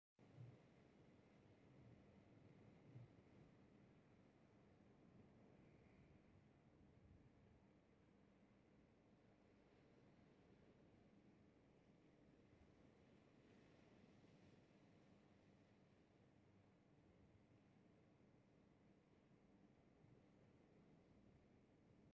International Film & Video
A bearded Moai statue sits on Easter Island on a hill above the ocean.